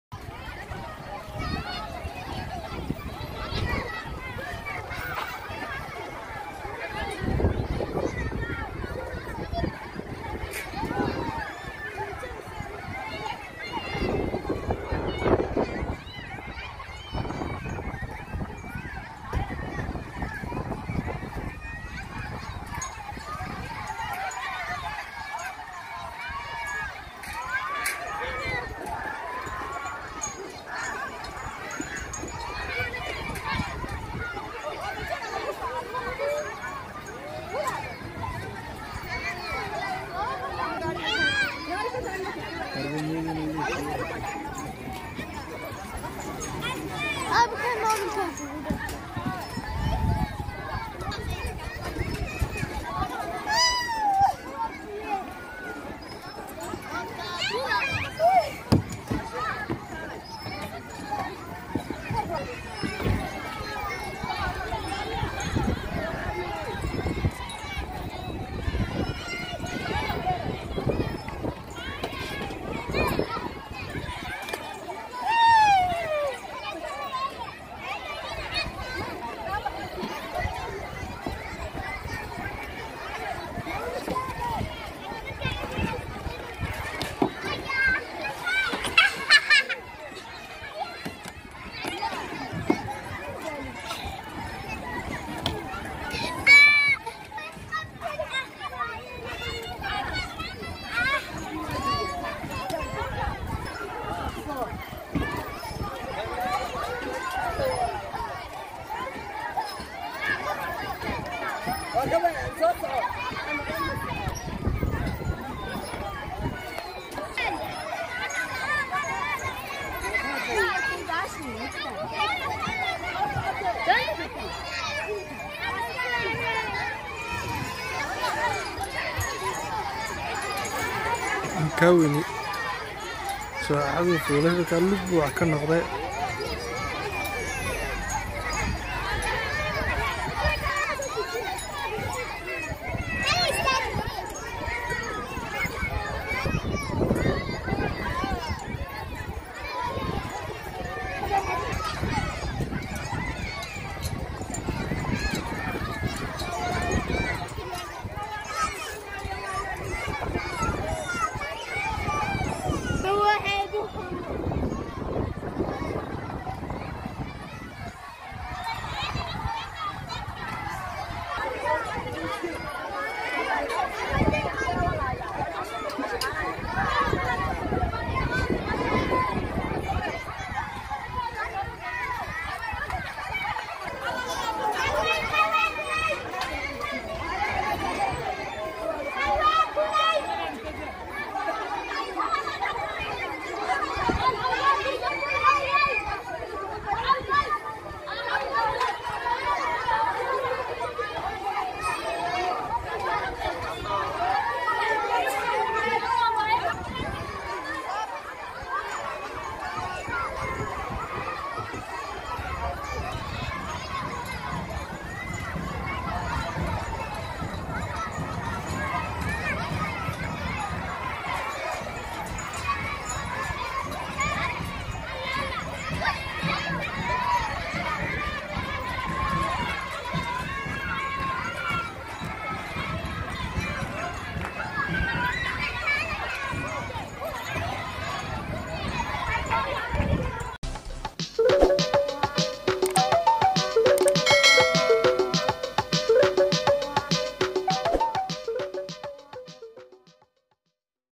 Children fun indoor the playground for kids and Family Africa Burco Farxadii Ciida Iyo Caruurtii
Children fun indoor the playground for Family
Farxada ciida halkay Ku ciyaaraan ciida oo hotel kaafi